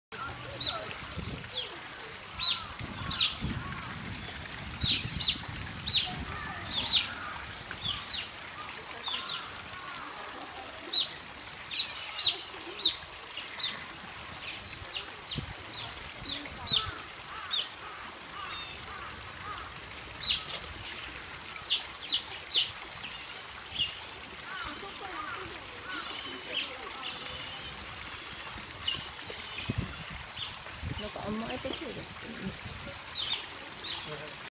コツメカワウソ 鳴き声
実は12種類も鳴き方のバリエーションがあるそうです。